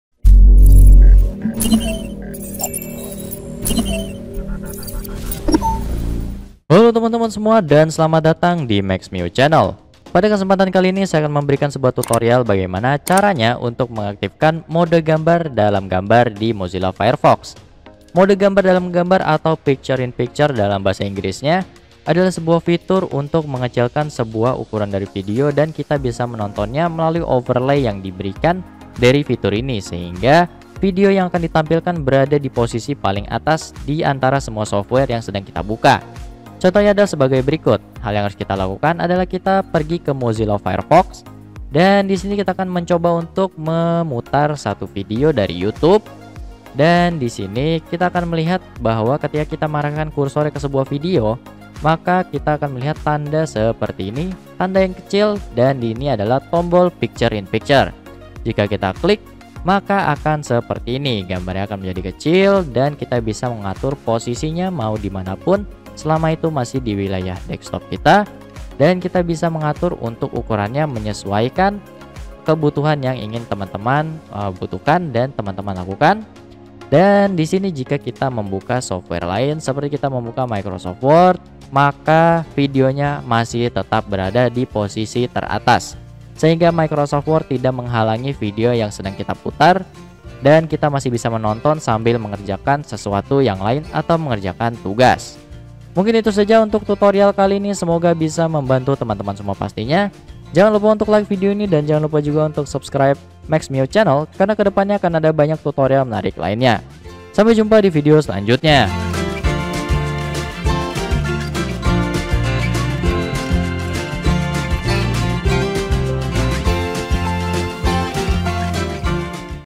Cara Menggunakan Picture in Picture di Mozilla Firefox
fitur pcture in picture ini memang sudah tersedia di Mozilla firefox tanpa harus menginstal add ons lagi seperti ketika menggunakan Google Chrome anda memang harus menginstal add on pic in pic.

Nah untuk Mozilla Firefox ini kita sudah bisa langsung menggunakanya, untuk anda yang belom tahu cara menggunakanya disini anda bisa simak video tutorial cara menggunakan picture in picture di Mozilla Firefox ini.

Untuk Google Chrome disini anda bisa simak caranya di video ini: Cara Menggunakan Picture in Picture di Mozilla Firefox

Daftar isi:
00:00 Intro
00:07 Pendahuluan
00:38 Tutorial Dimulai